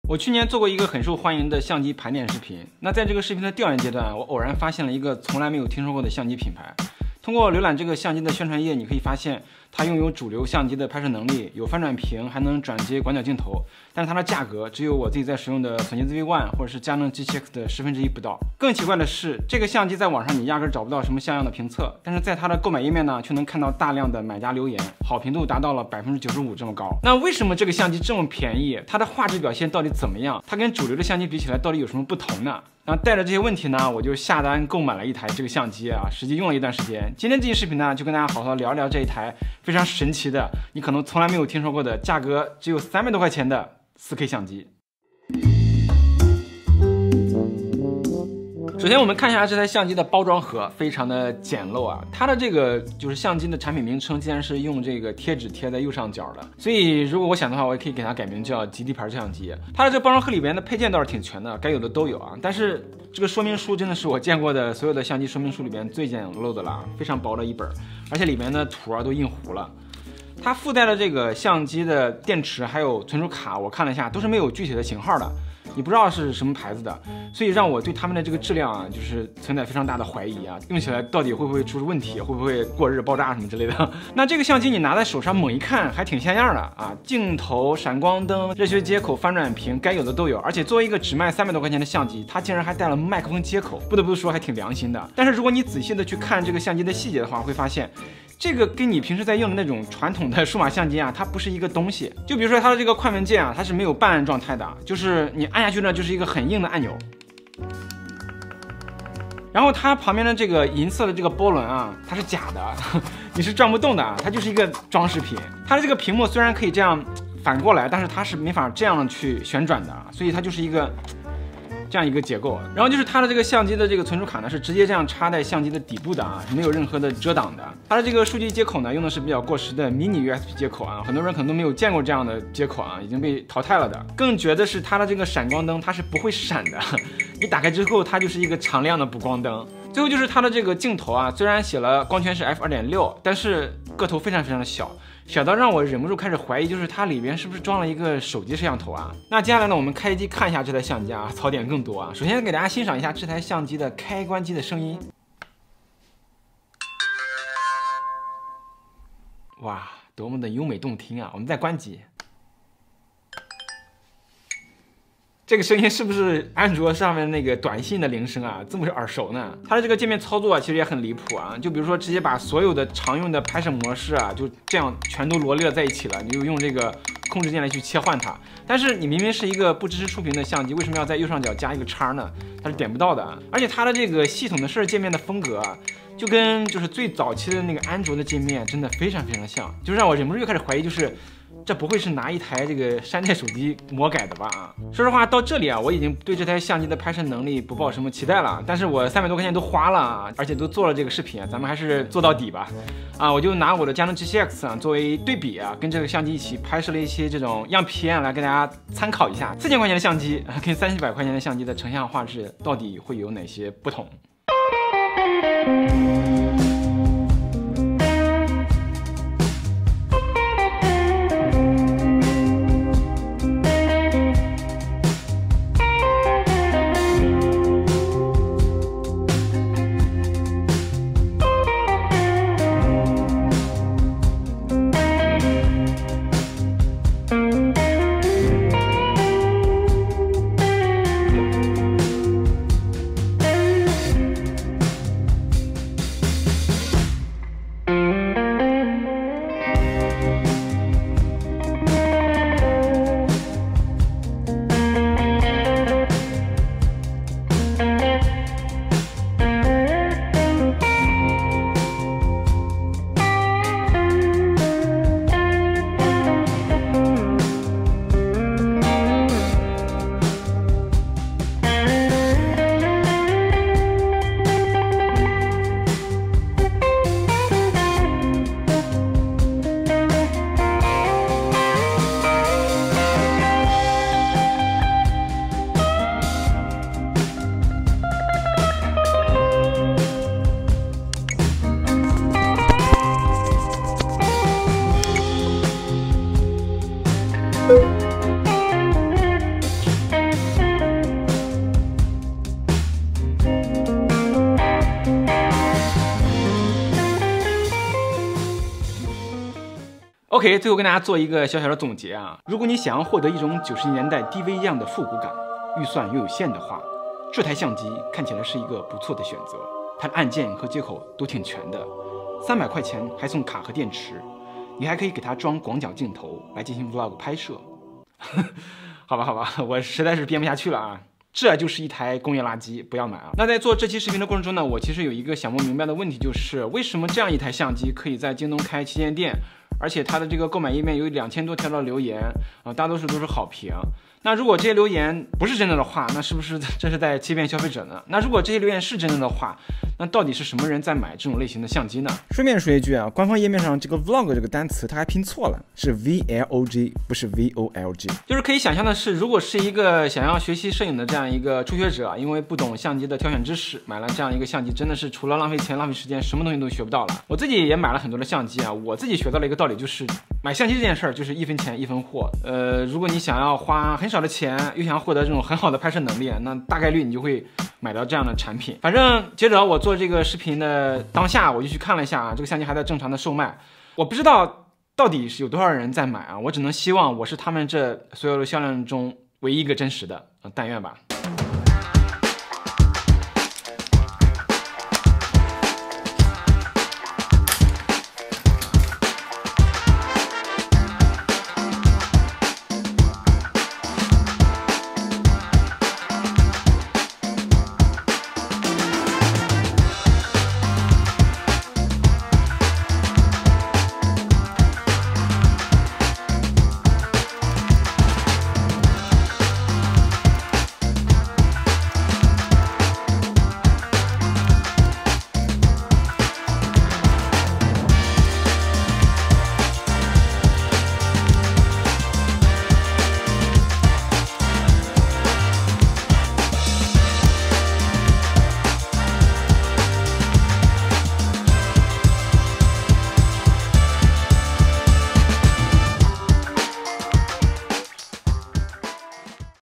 300元的入门4K相机可以买吗？（对比佳能G7X3）by 极地手记
300块钱就能买到的4K相机用起来到底怎么样？今天这期视频，就跟大家聊聊这台非常神奇的你可能从来没有听说过的相机品牌